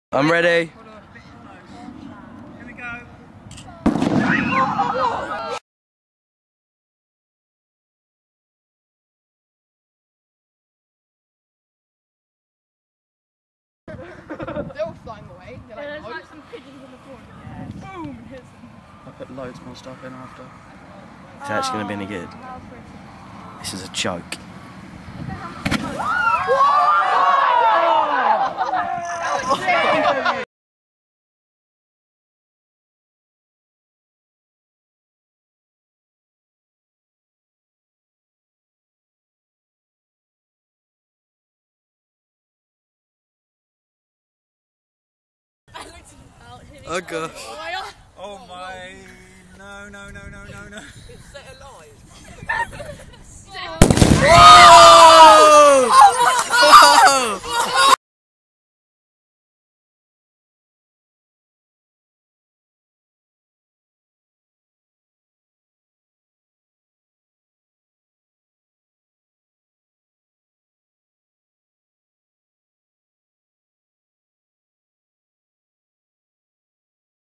Melon and Stars
Year 9 Special Effects added to a melon and Star effect pyrotechnics